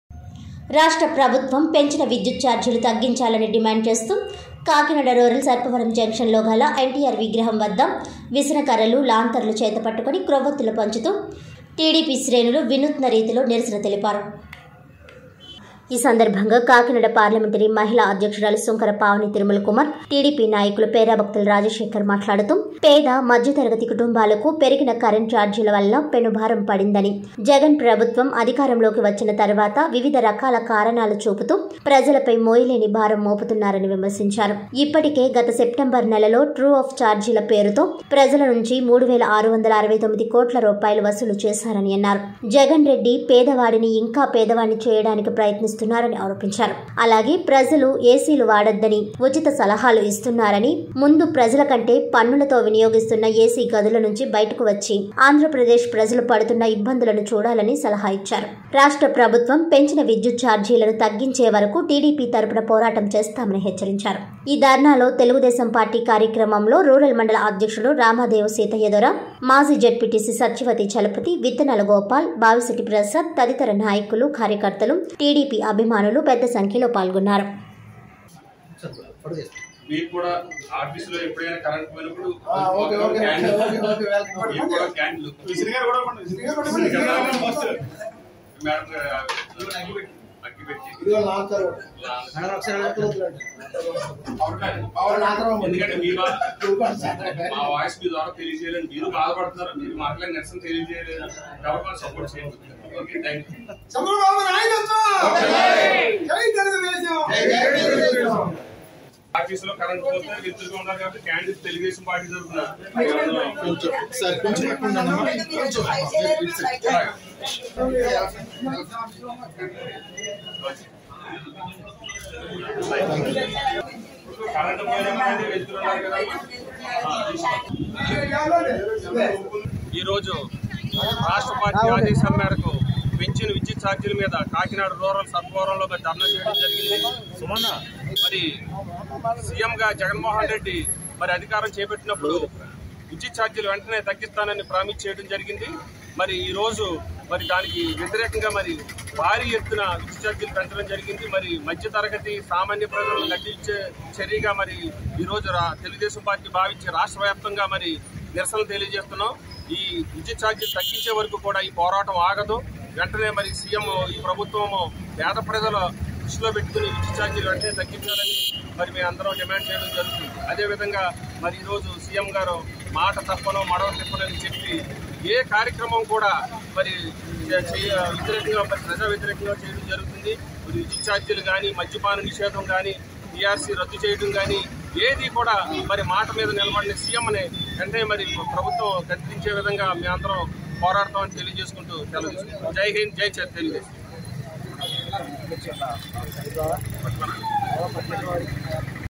కాకినాడ రూరల్-|04-03-2022|TV 7 NEWS
రాష్ట్ర ప్రభుత్వం పెంచిన విద్యుత్ చార్జీలు తగ్గించాలని డిమాండ్ చేస్తూ కాకినాడ రూరల్ సర్పవరం జంక్షన్ లోగల ఎన్టీఆర్ విగ్రహాం వద్ద విసనకర్రలు, లాంతర్లు చేత పట్టుకుని కొవ్వత్తులు పంచుతూ టిడిపి శ్రేణులు వినూత్న రీతిలో నిరసన తెలిపారు.